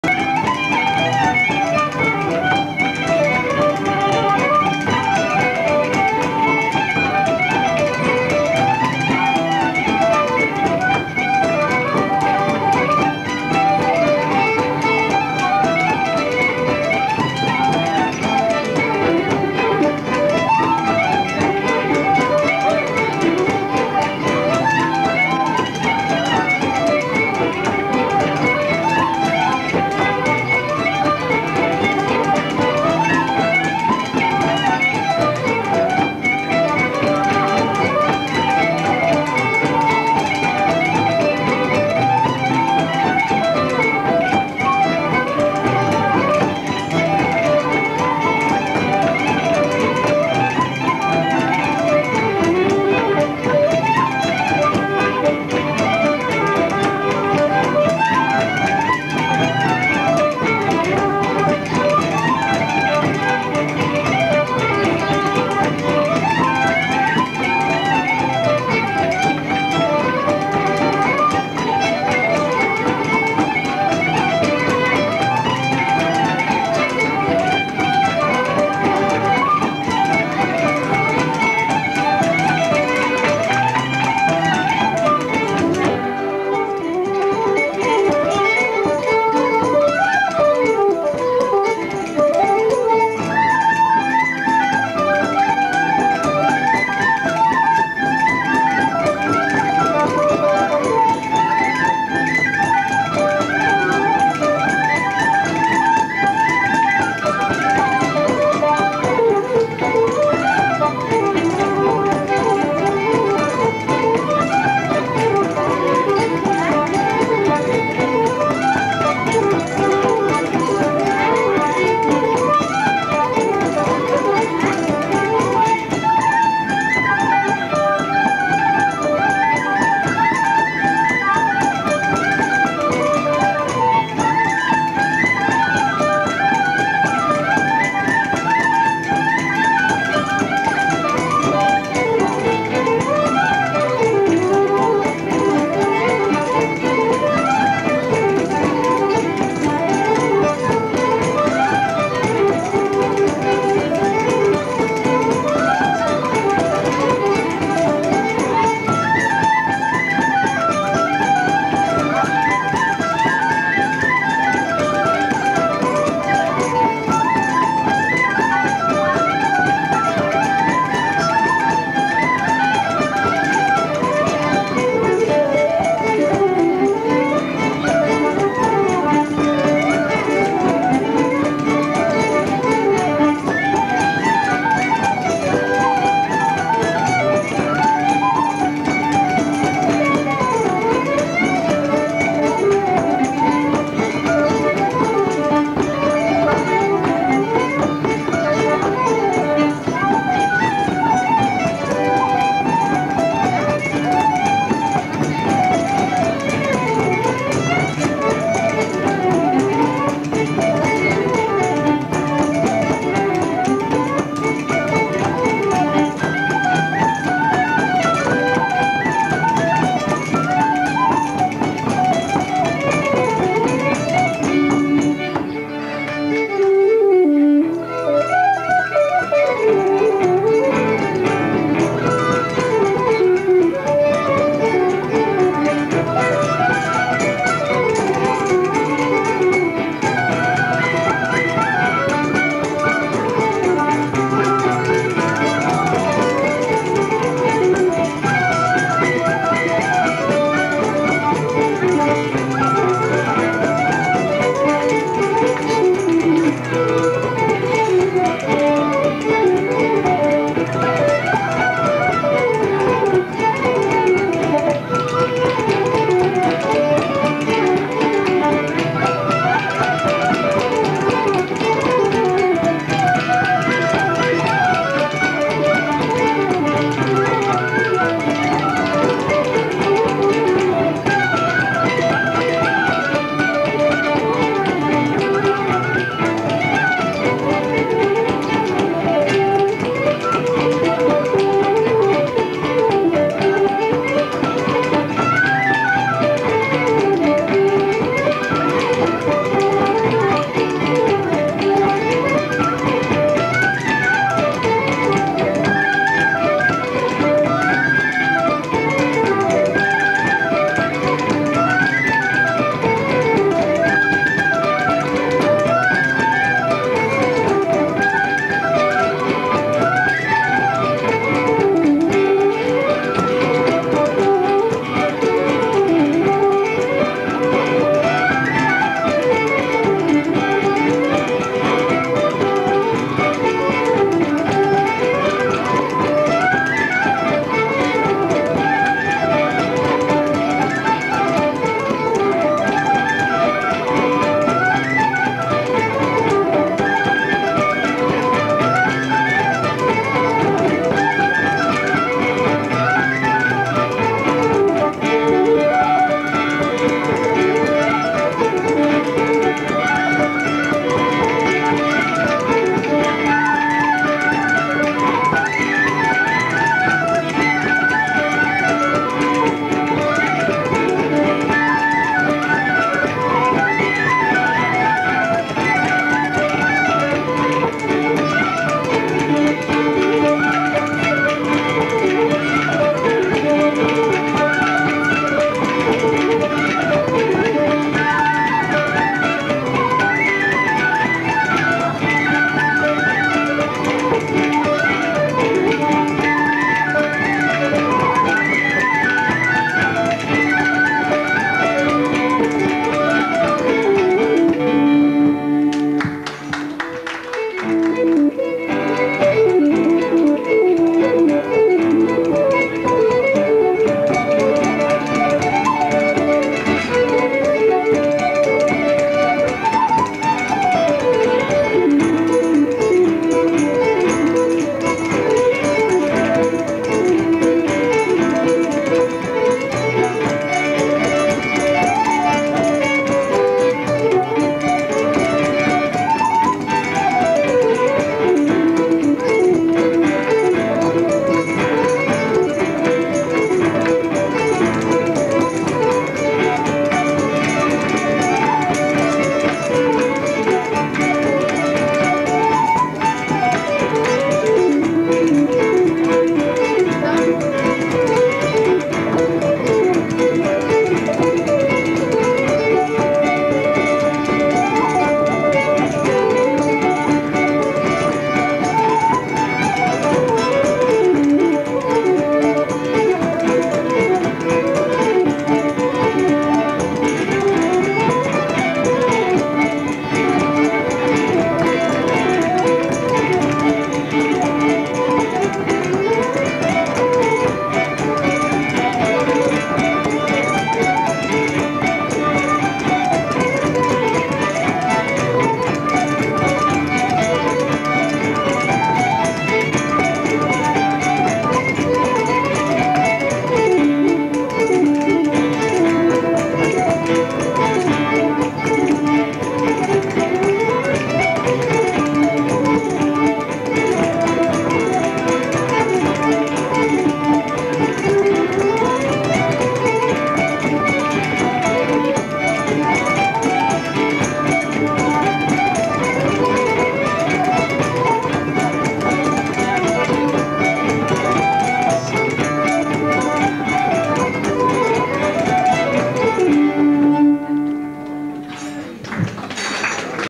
Gurteen Session 1992.Part 1
Session in Teach Murray Summer 1992
Musicians. Peter Horan Tommy McGowan Noel Tansey Joe Stenson Phil Cunningham Brian McGrath Caroline and Brian Garvin Sonny Davy Sandy Percival.